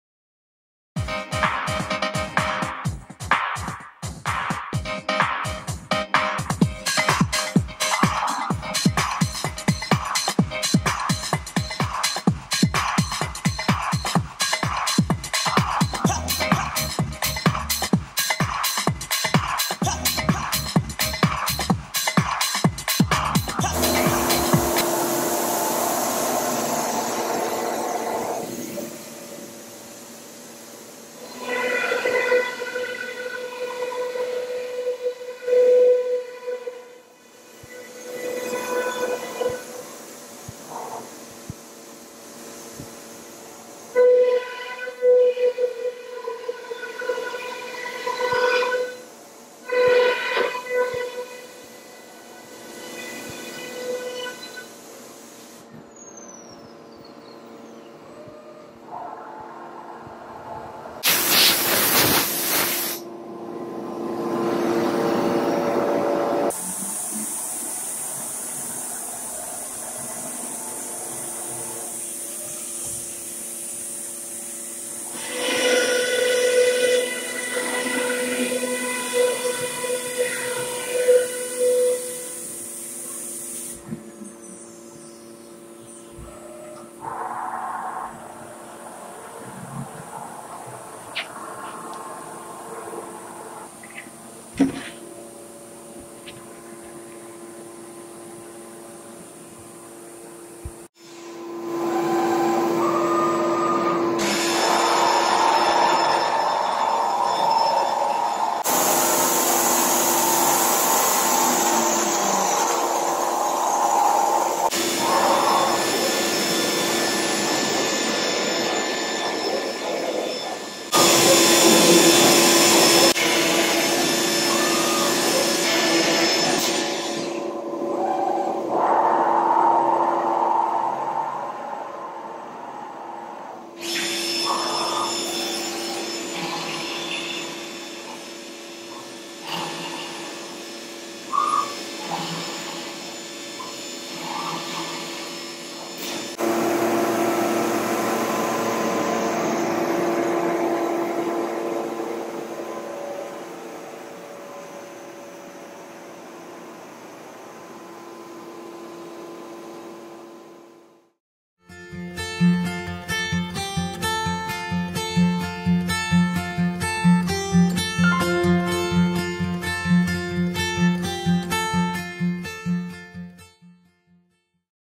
SCM Tech99 CNC Router with ATC - Cabinet Maker
* SCM TECH99 - Pod & Rail -  CNC  Cabinet Maker - Year 2002
* Working Length: 118" Working Width" 39"
* 5 head ATC with 5 Holders and tools on rack and
* Vacuum Table with pods.
* Bullard Model 41A Airline Filter.
* Vacuum Pump Rietschle Druvac -
* Voltage 400, 3 Phase - Client does not have 3 Phase, 400 Volts and runs it off Single Phase 240 Volts supply, so has a Transformer & Phase Convertor - Both for $7,000 if needed.
* Drill Bank with a total of 20 bits, Vertical & Horizontal for surface and end side holes.
* Comes with Computer, New Computer tower,
* 2 and design softwares, XYlog and Polyboard, comes with.
* Used 2 x a week for 4 – 5 hours only.
* Under power - Good running condition.
Location: Windsor, Ontario
Reason for Sale - Retiring.
Call Jim - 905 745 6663